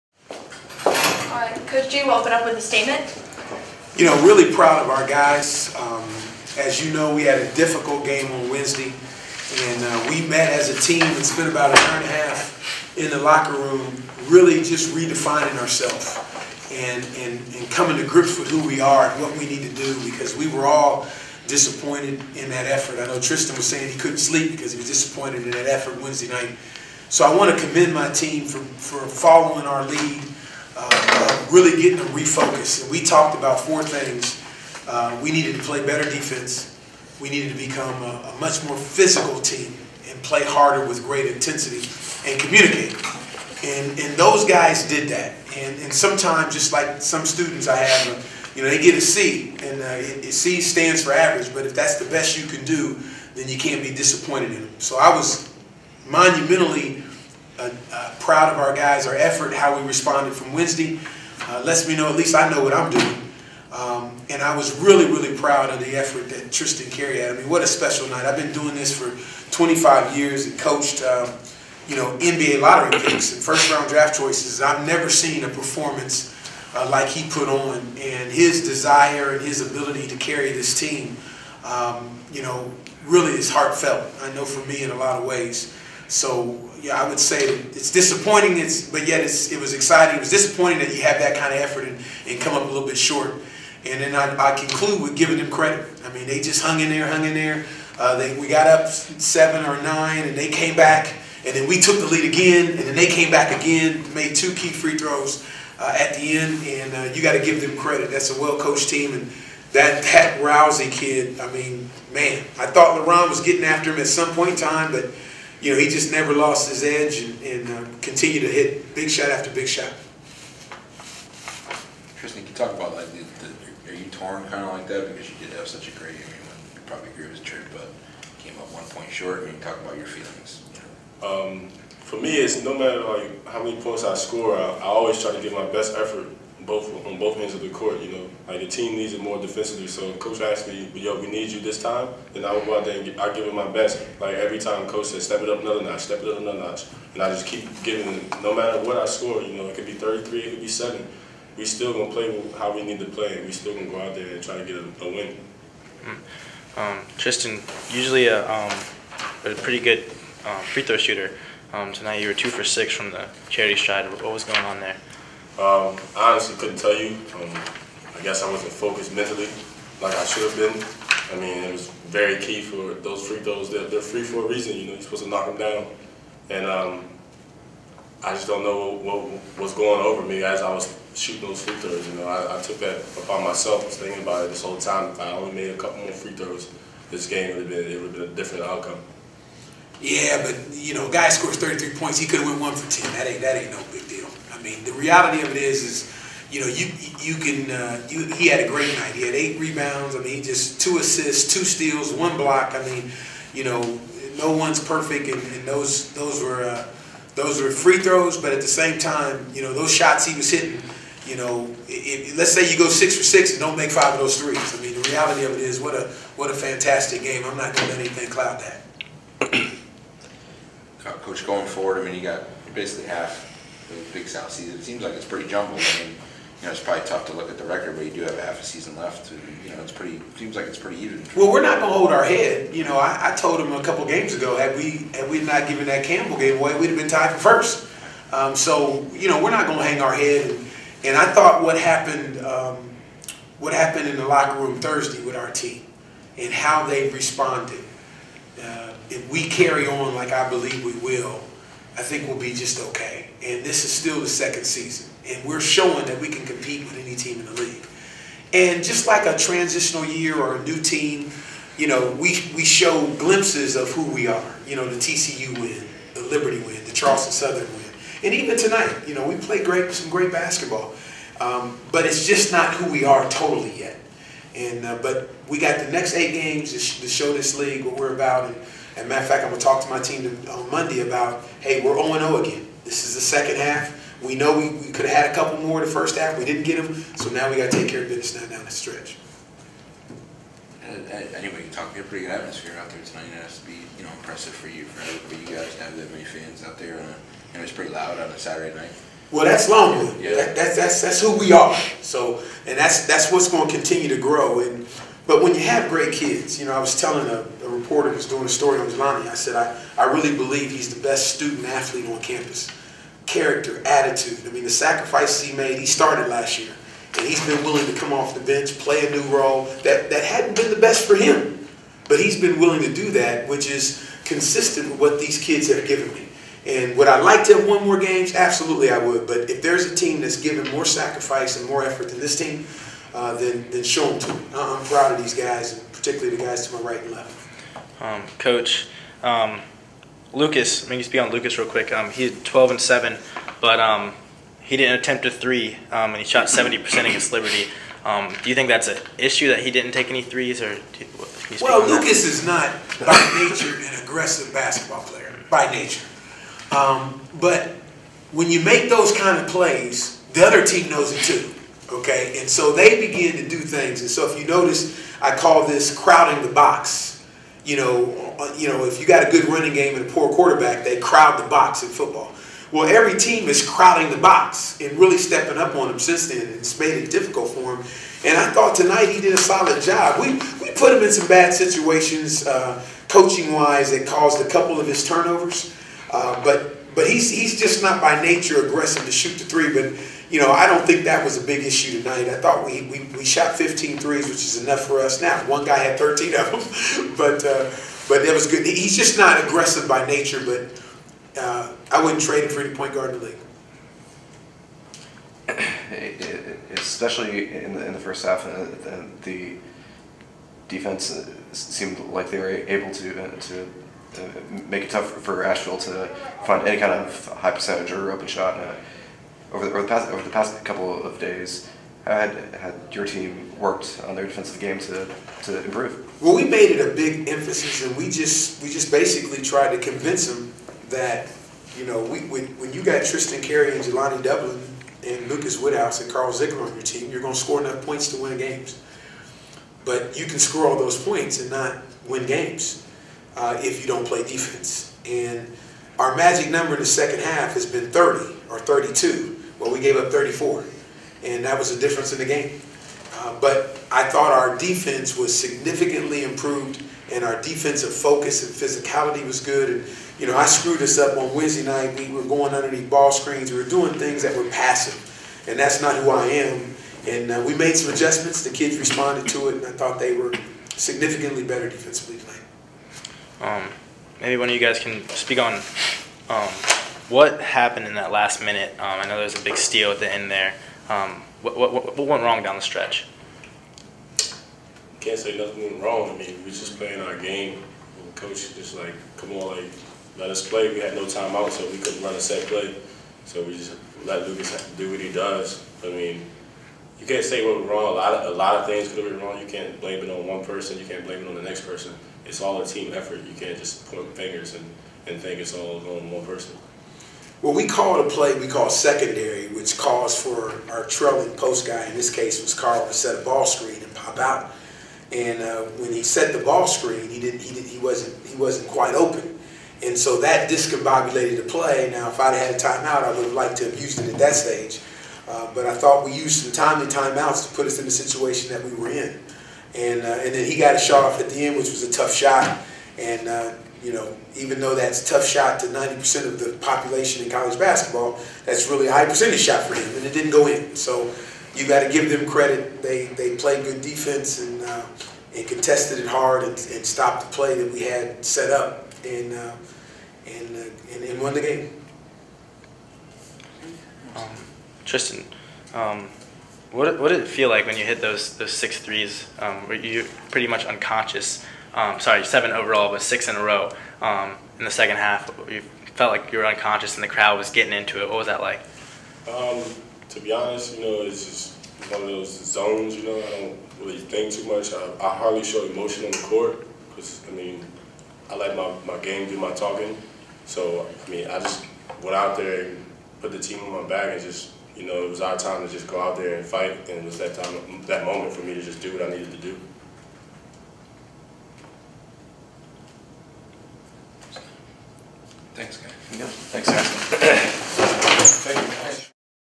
Longwood Men's Basketball press conference 2/1/14
Join the UNC-Asheville post-game media press conference and hear from Coach Gee, Jeylani Dublin, and Tristan Carey.